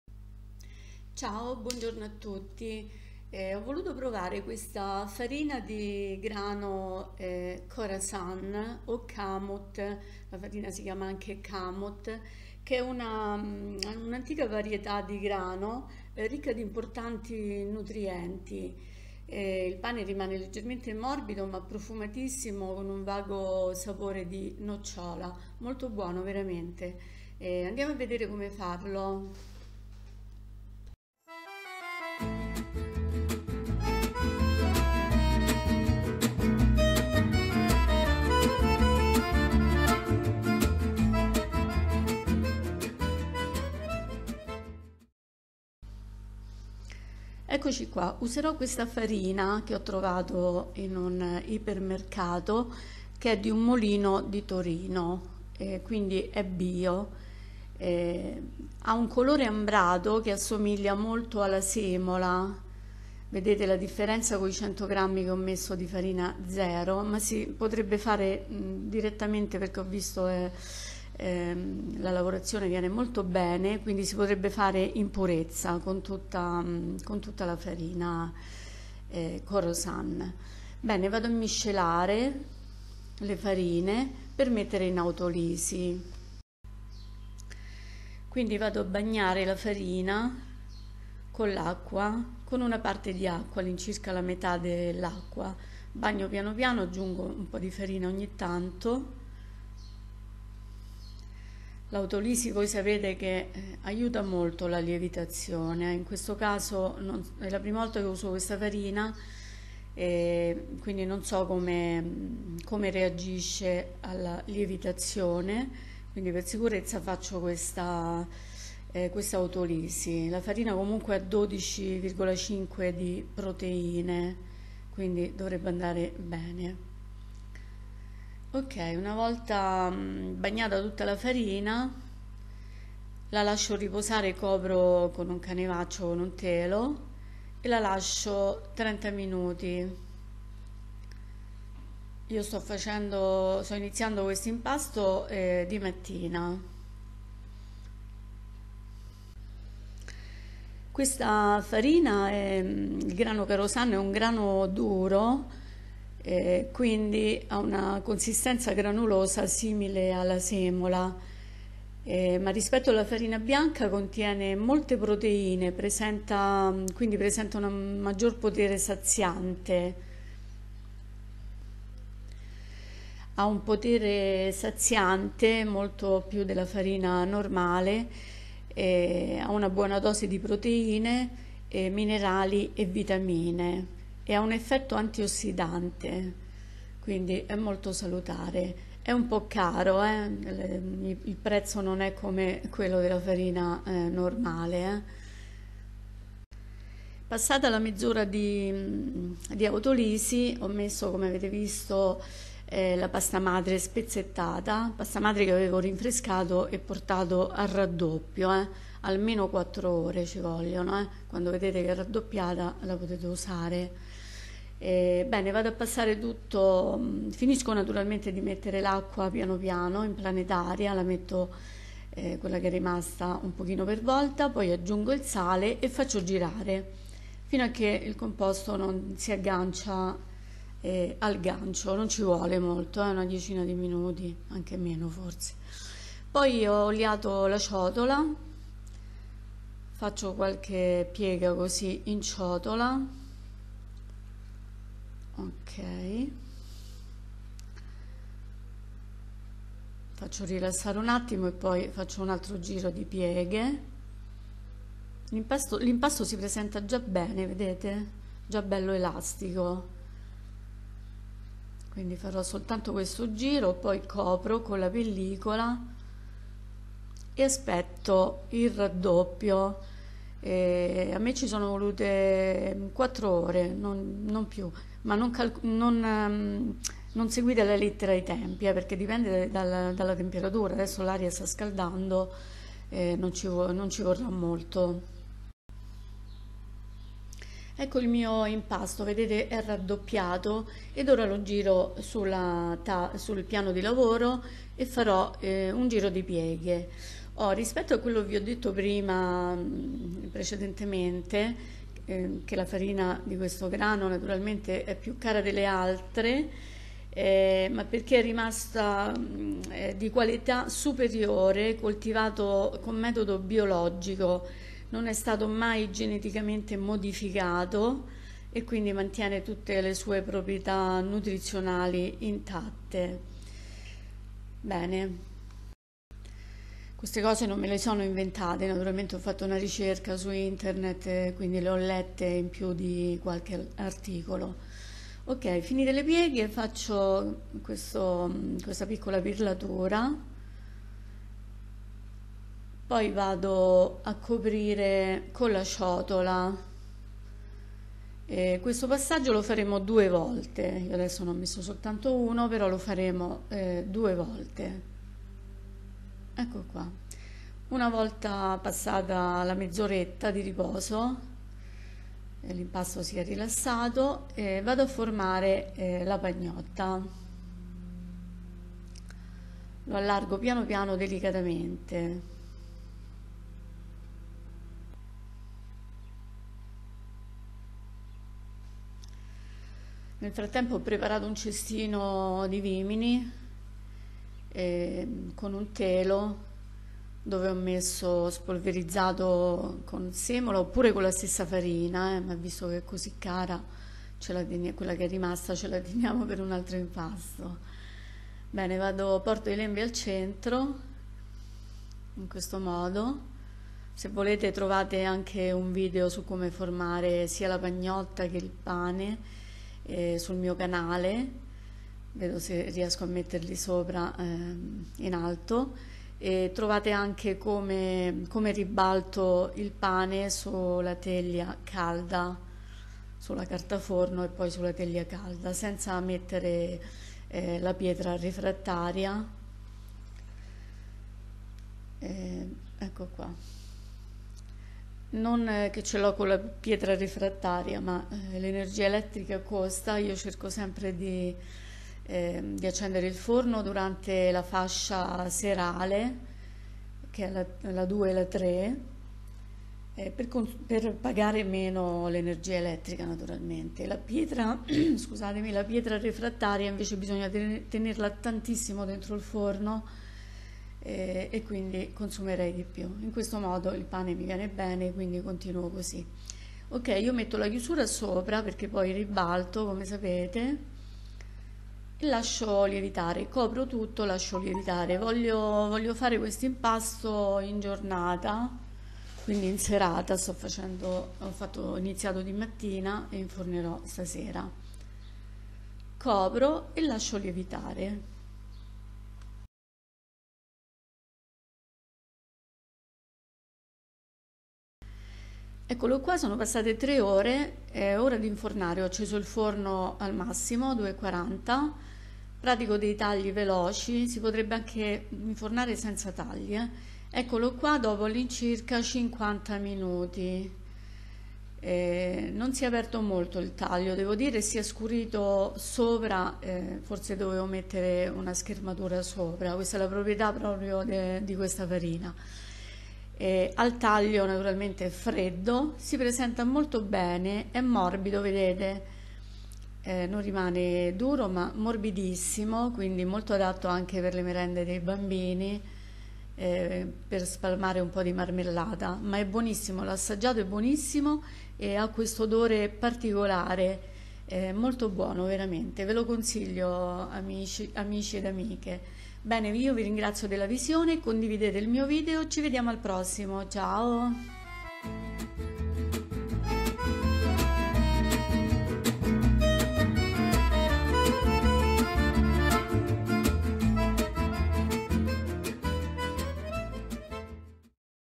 Pane con farina Khorasan (Kamut) con lievito madre
Pane con farina Korasan con lievito madre
errata corrige: naturalmente alla farina aggiungo acqua non farina!
scusatemi...